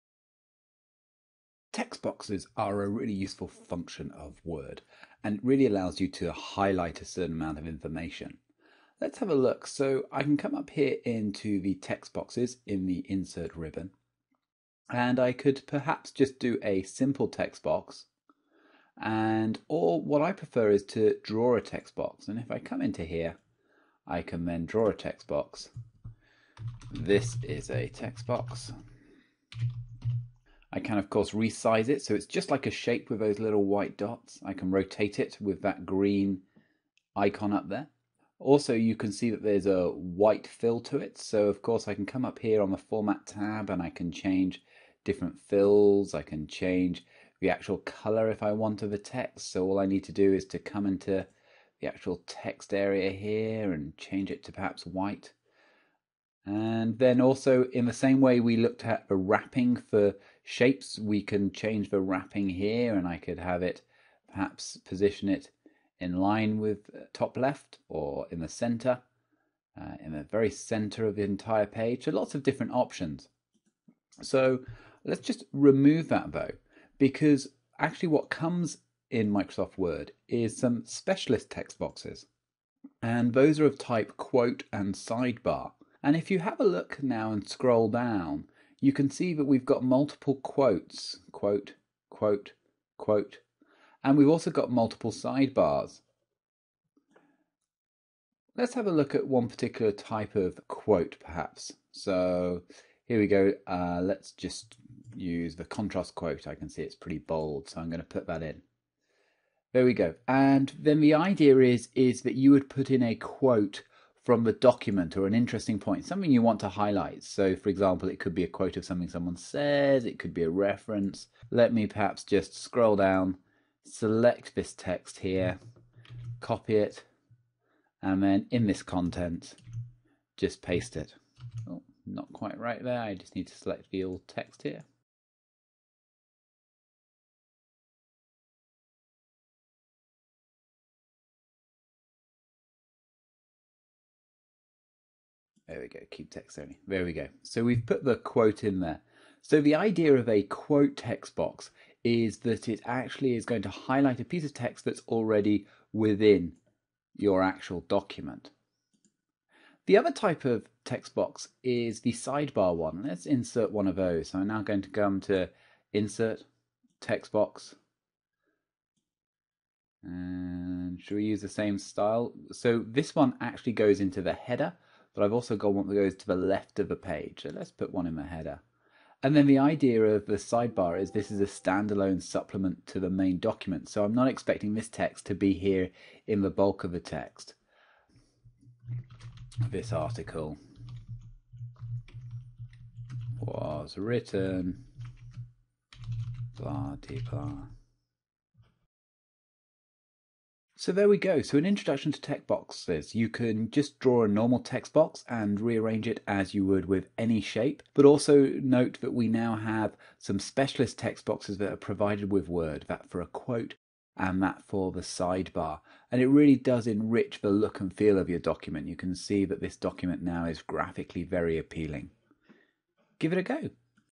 31. Word - Text Boxes - simple, quote and sidebar
Word - Zero to Hero - Text Boxes

This video is part of the free online courses available - provided by Learning Specialists